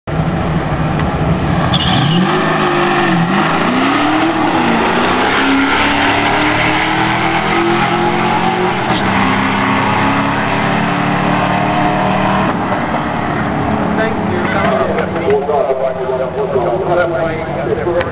racing
94 lt1 camaro against bolt on slicks geared gt mustang